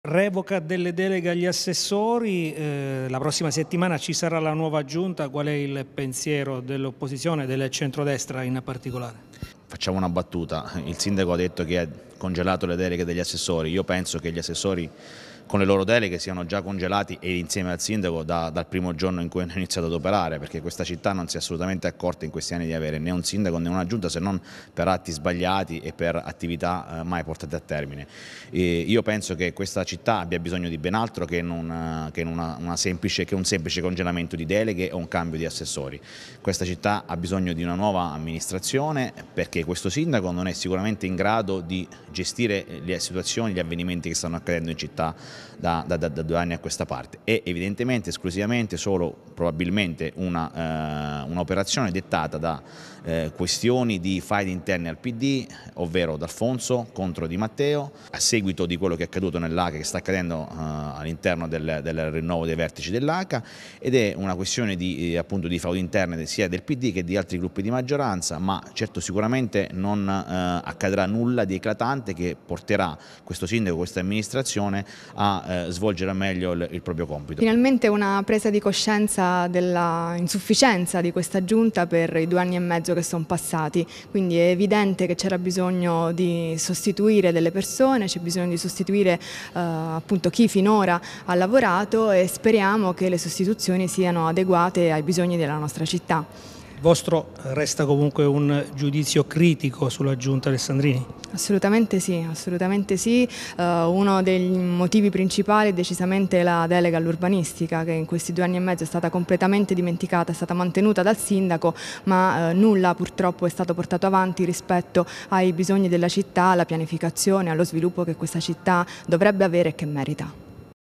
Le opposizioni a Pescara sul rimpasto di Giunta
Centrodestra e M5S parlano del rimpasto di Giunta a Pescara, giudicando negativamente gli oltre due anni di amministrazione